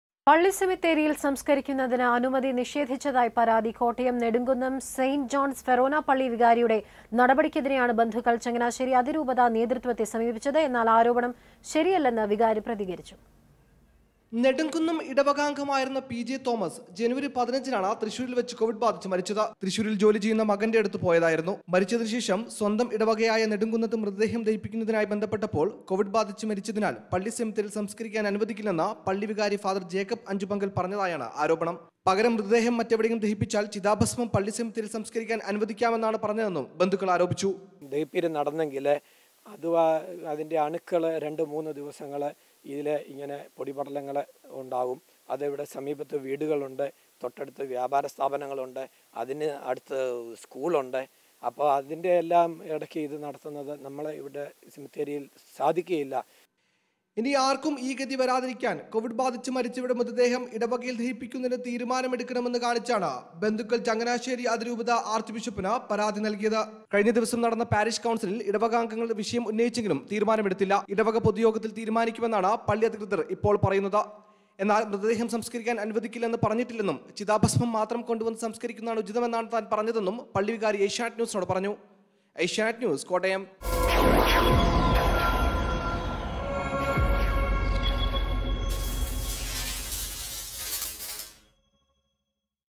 'കൊവിഡ് രോഗിയുടെ മൃതദേഹത്തോട് അനാദരവ്'; അതിരൂപതയ്ക്ക് പരാതി നല്‍കി ബന്ധുക്കള്‍
Dishonouring dead body of man died of covid